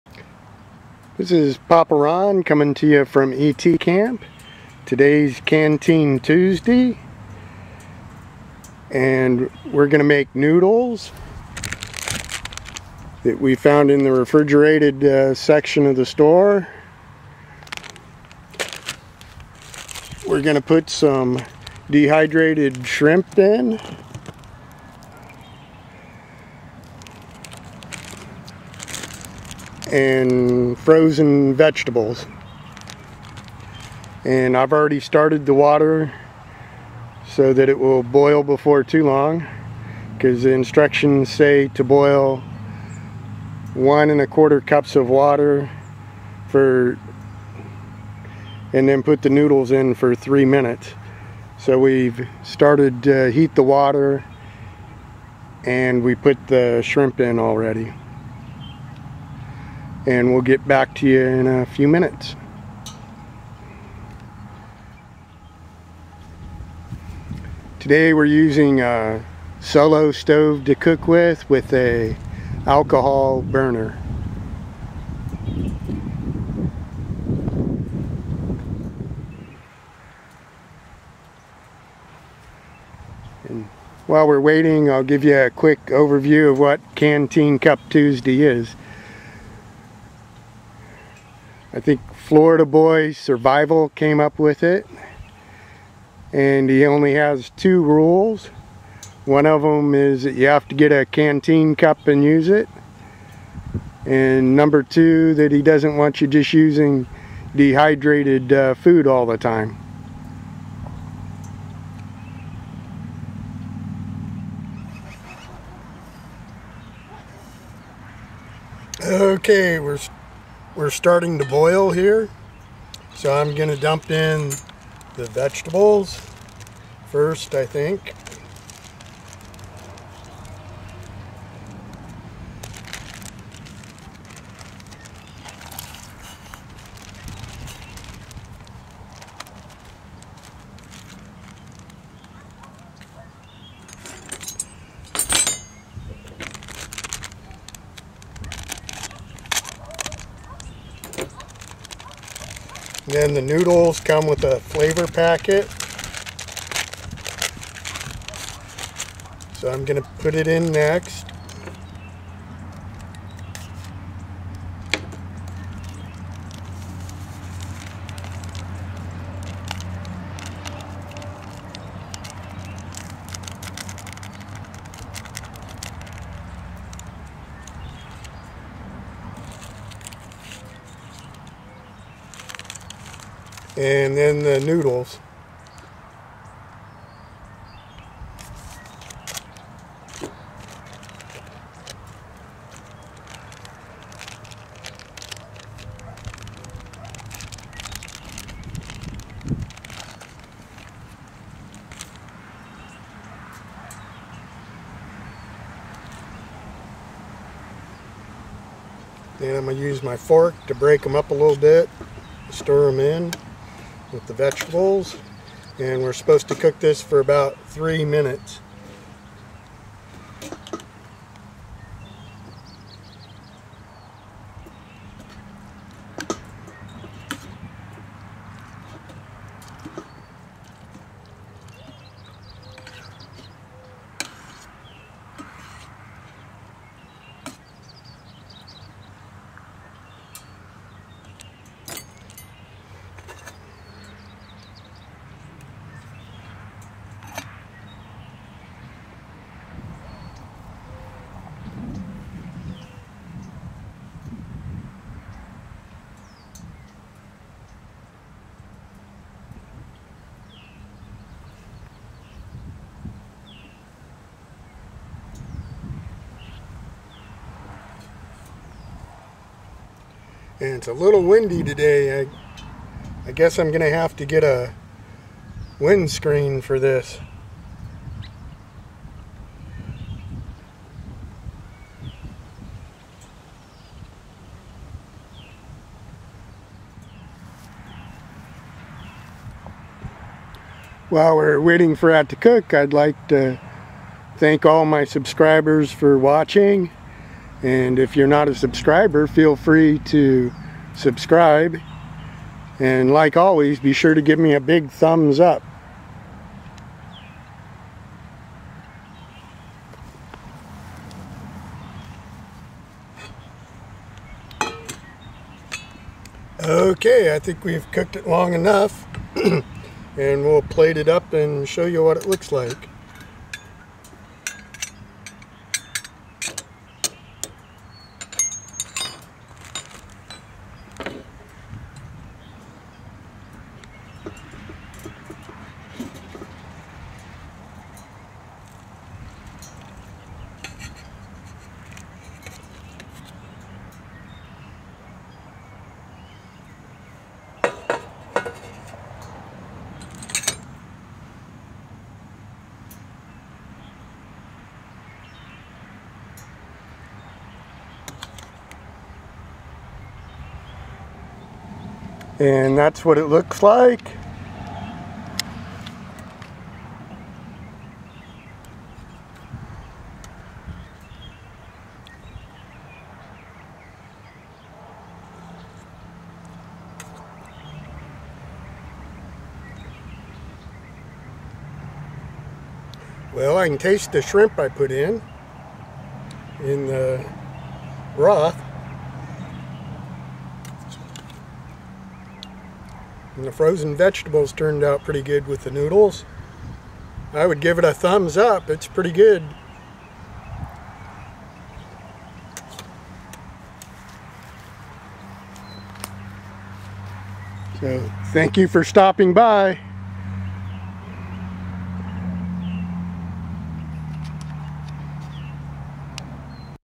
Canteen Cup Tuesday Noodle Soup PaPa's Page
Canteen Cup Tuesday Noodle Soup
PaPa's Page, Cooking with a canteen cup